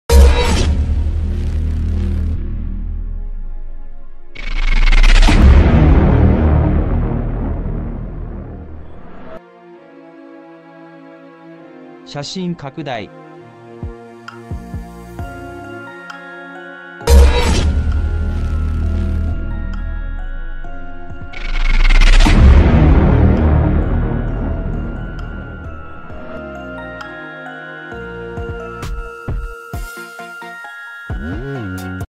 関根潤三さんが死去 球界関係者の追悼コメントTop News
,News,関根潤三さんが死去　球界関係者の追悼コメントTop
関根潤三さんが死去 球界関係者の追悼コメントTop News 写真拡大